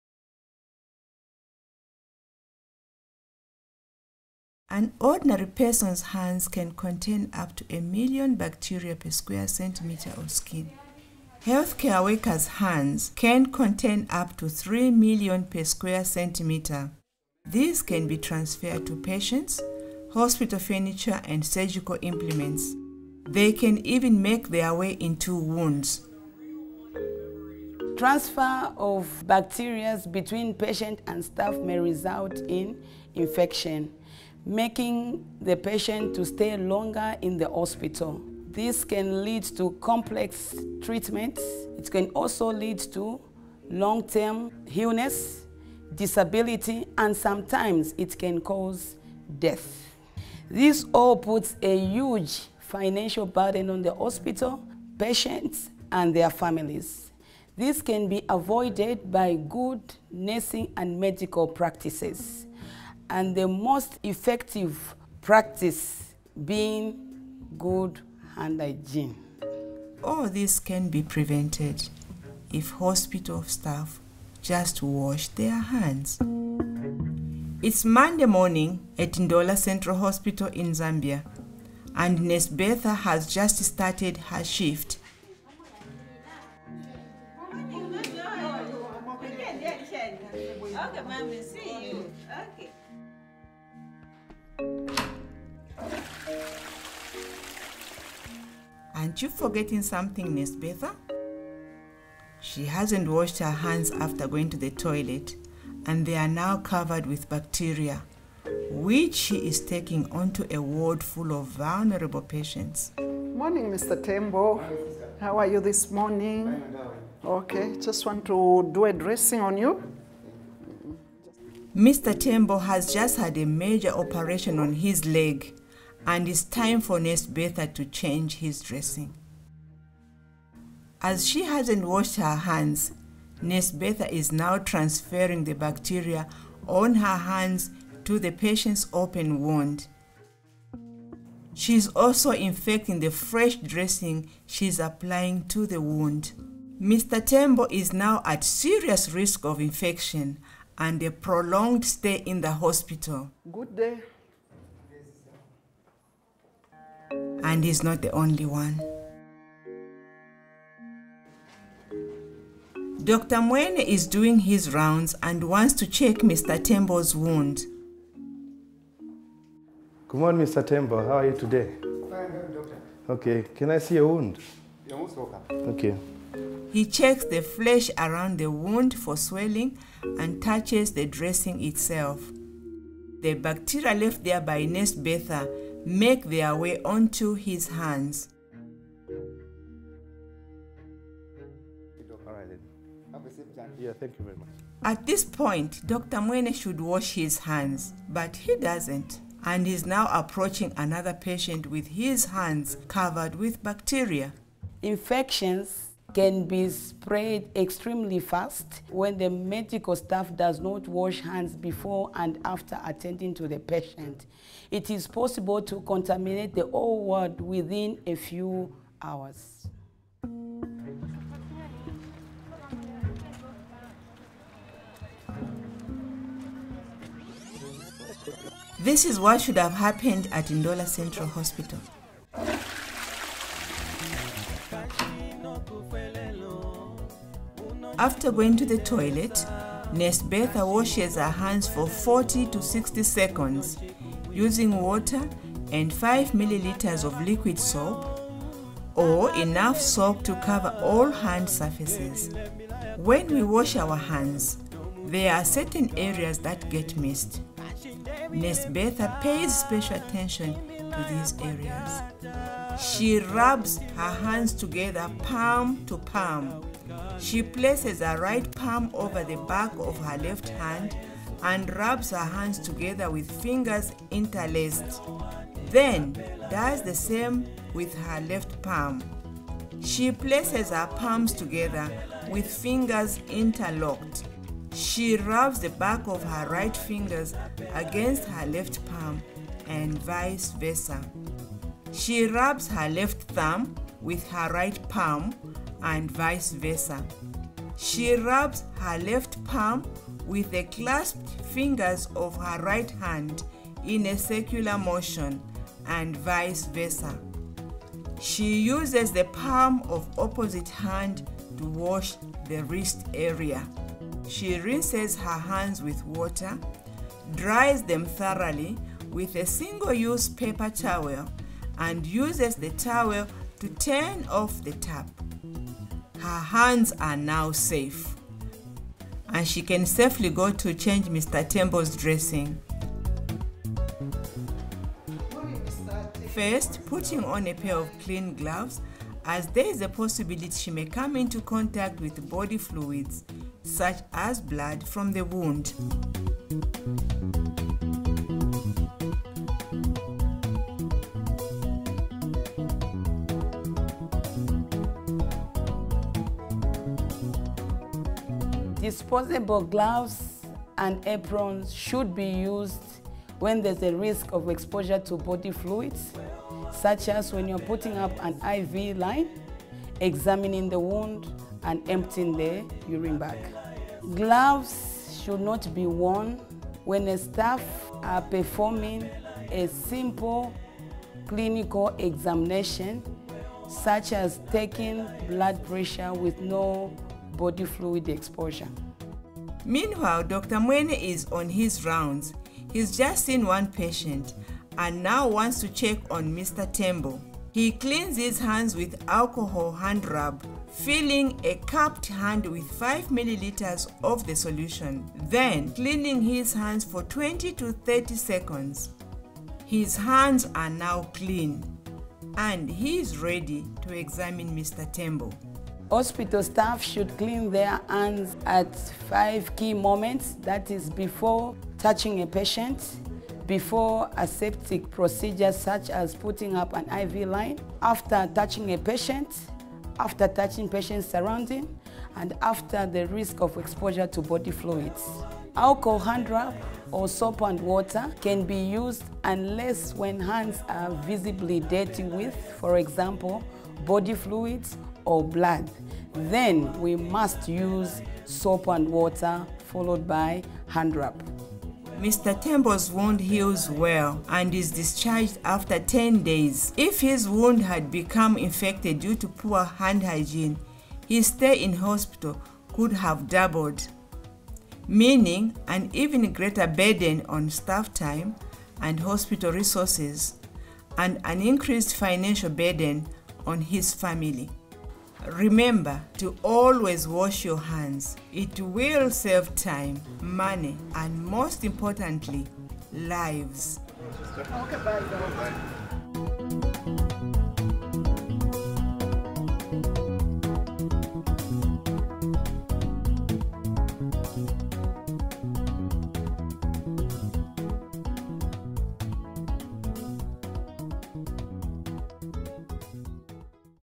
Handwashing in Hospitals
This film covers handwashing best practice for hospital staff and health workers.